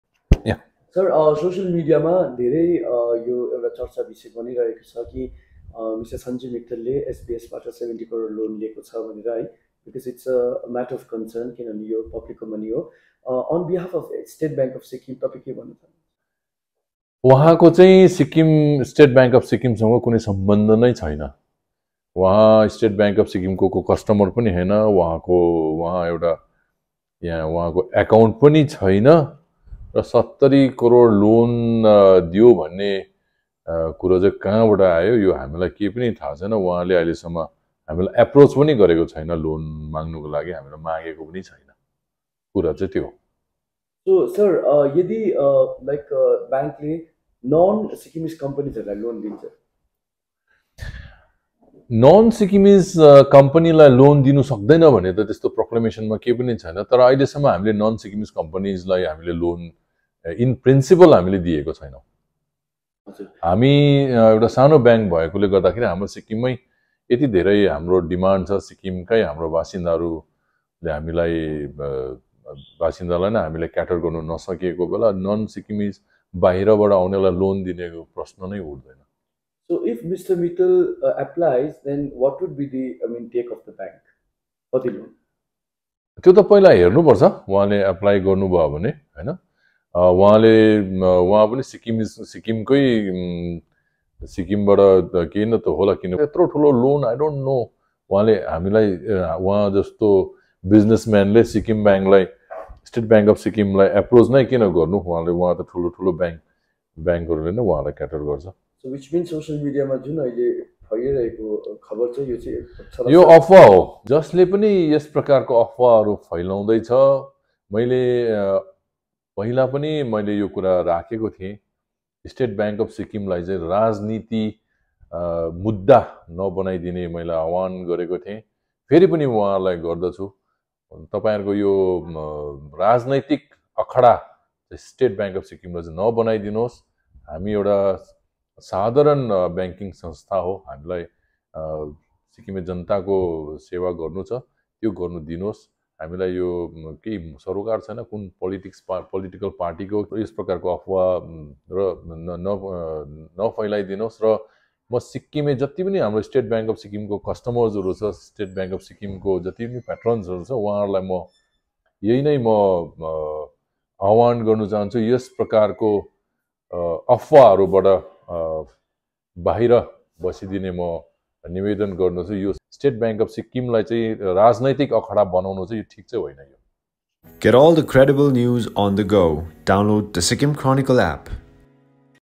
SBS has refuted the allegation of businessman Sanjay Mittal taking a loan of 70 crore from the bank
sikkimchronicle | NEWS

State Bank of Sikkim has refuted the allegation of businessman Sanjay Mittal taking a loan of 70 crore from SBS.

" Forget Loan he even doesn't have account with our bank. Please don't politicise our institution " Mangaing Director, Phurba Wangdi Bhutia.